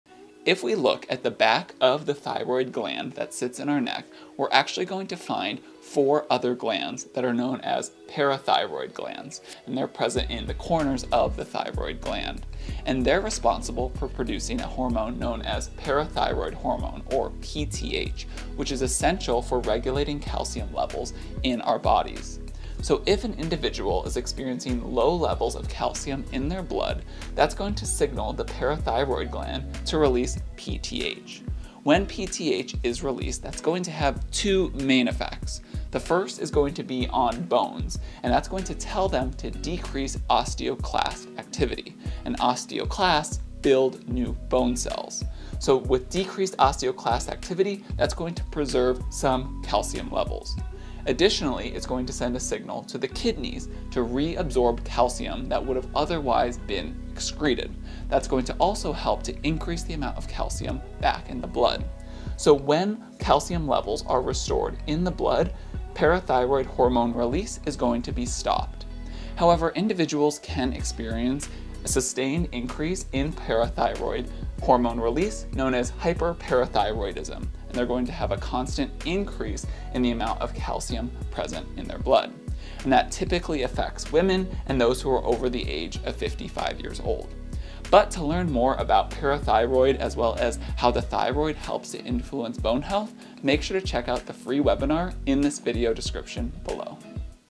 Parathyroid and PTH Overview
The parathyroid glands are four small endocrine glands located behind the thyroid gland in the neck. They produce parathyroid hormone (PTH), which regulates calcium levels in the blood by increasing calcium absorption from the gut, releasing calcium from bones, and conserving calcium in the kidneys. Proper functioning of these glands is crucial for maintaining stable calcium levels, which are essential for nerve and muscle function.